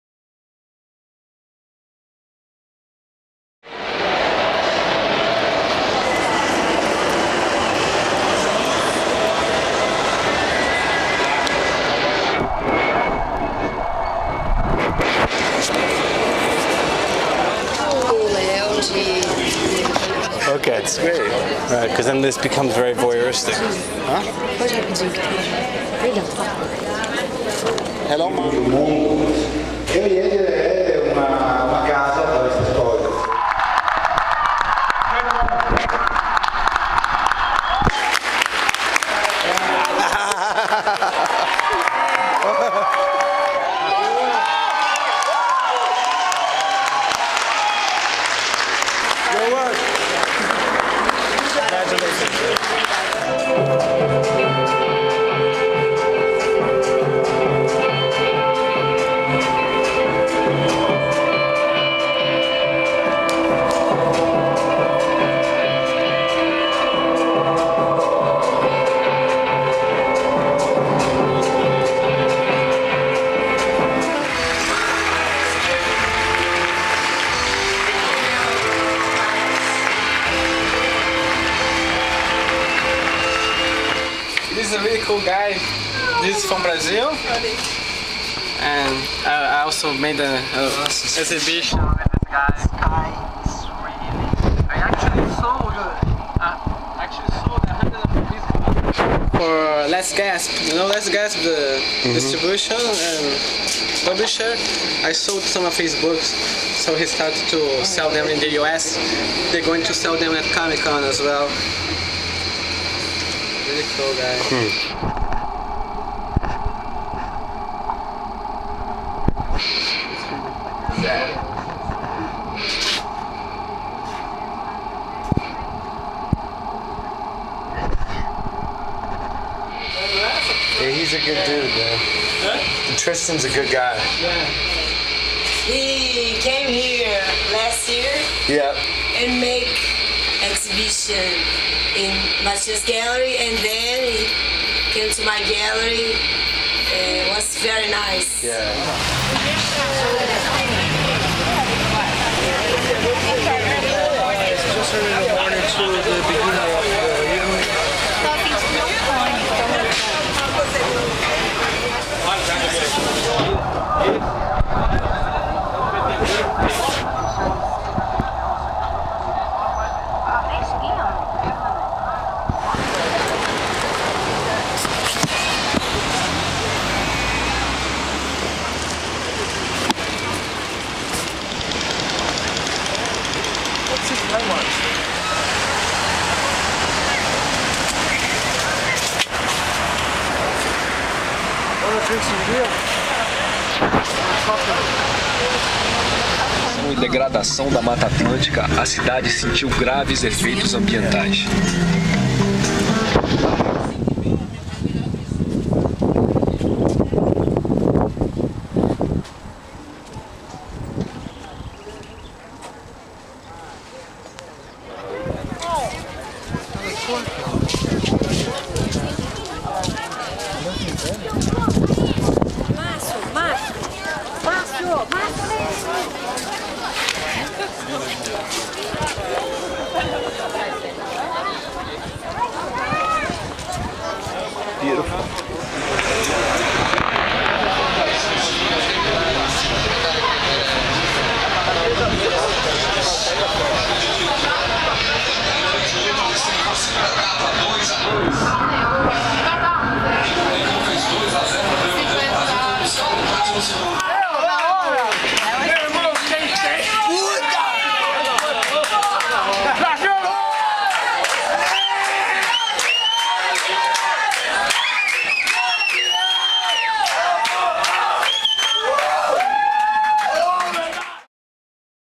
BEINGMARCECKO.com- BRAZIL June 2009-Sao Paolo Fashion week
sao paolo fashion week- a vist to Alexandre Herchcovitch's show- visiting the "christo"- brazil beats USA 3-2-- RIO